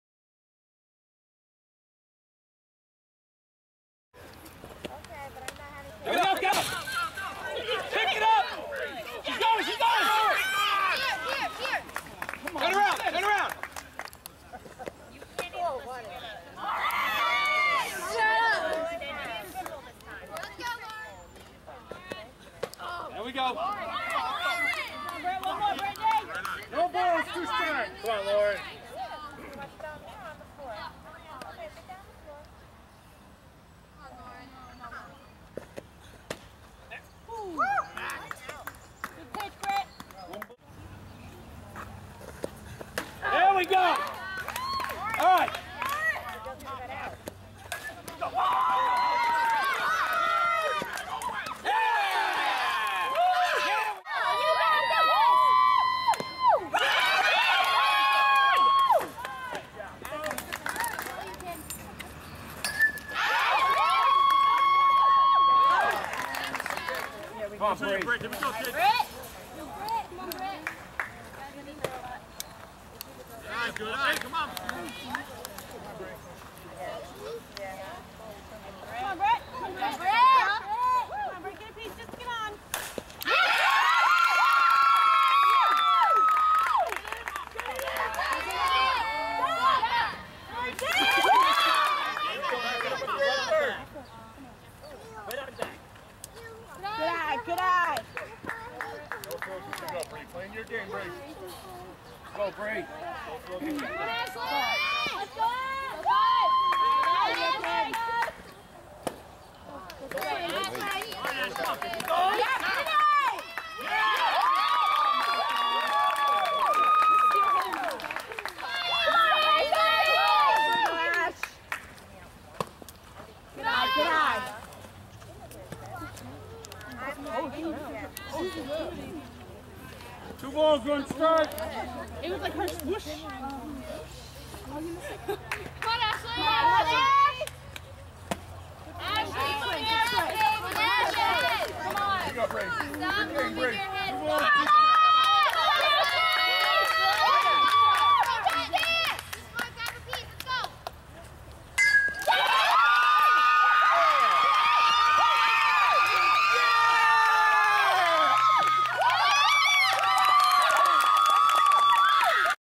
MW Little League Real Time vs IPros playoff 6-19-2012
This video is about the monroe woodbury little league. Great game by both teams.